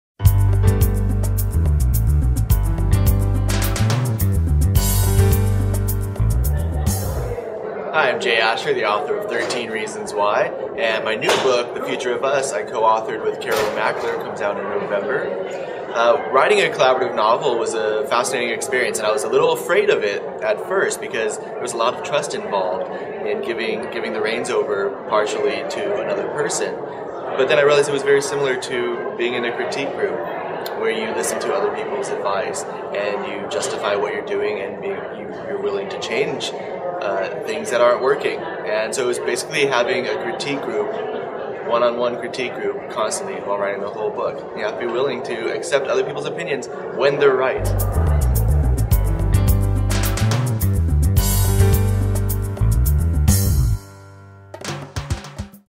Jay Asher
YA author of Thirteen Reasons Why, Jay Asher talks about the collaboration with
 Carolyn Mackler. on their book, The Future of Us from Razorbill,